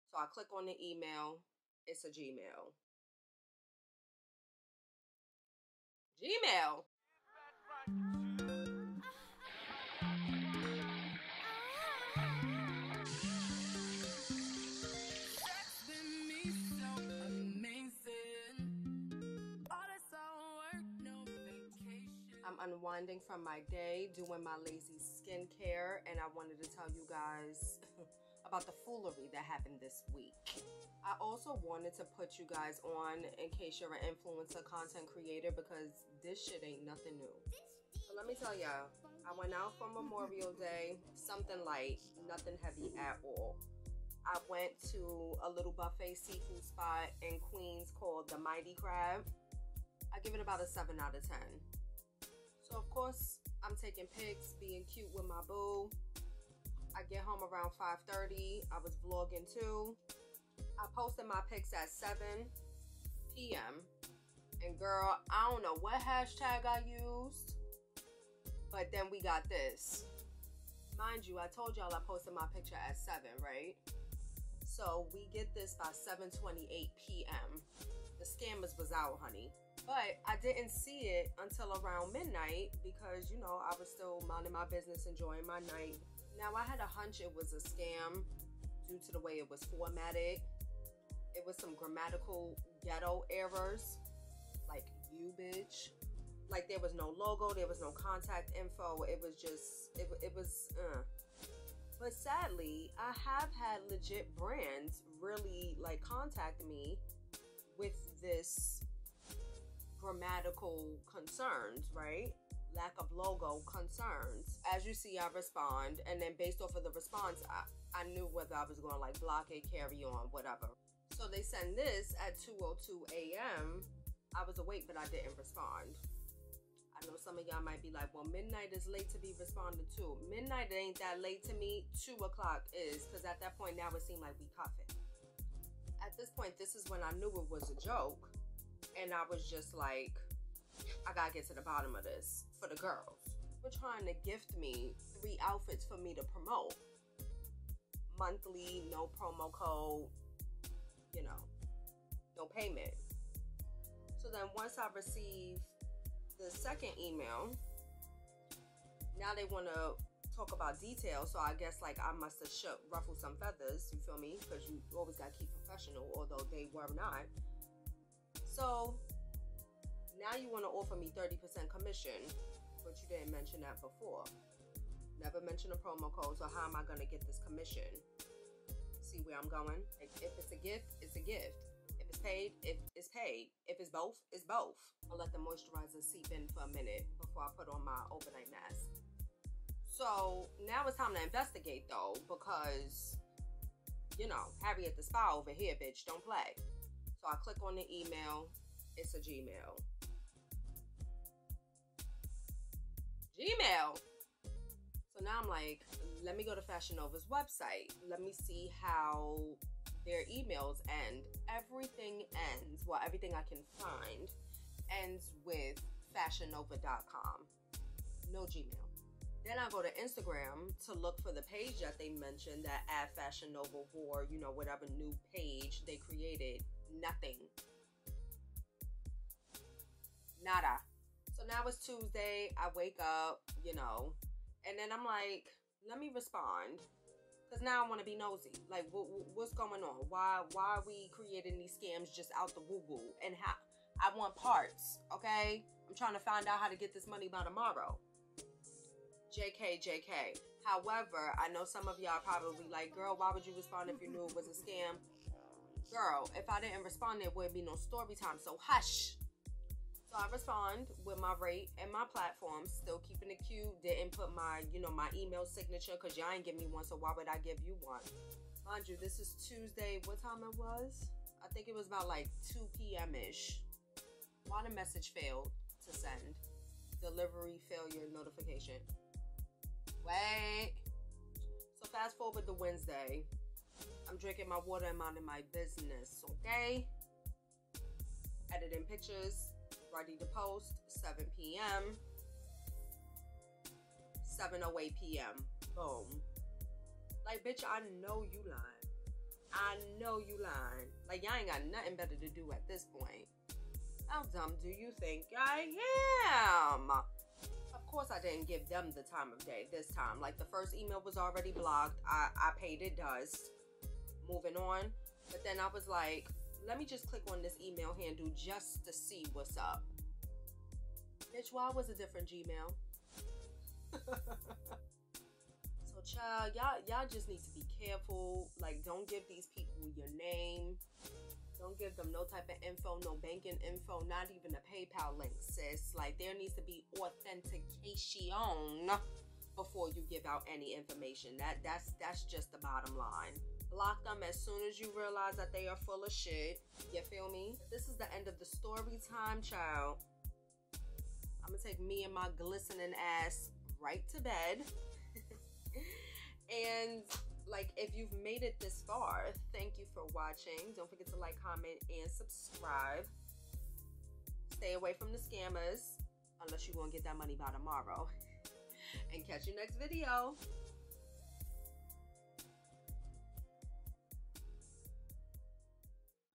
Storytime Fashion Nova Scam 👀 𝘄𝗵𝗮𝘁 𝘁𝗼 𝗹𝗼𝗼𝗸 𝗳𝗼𝗿!
Hey y’all hey!

The 𝗧𝗘𝗔 on what influencers need to look for with these “collaborations” 👀

XOXO,
STALEYINSIGHT 💋

Follow Me To Keep Up With Your Girl!

⇢ Location: New York, NY
⇢ Height 5' 2"
⇢ Race: Black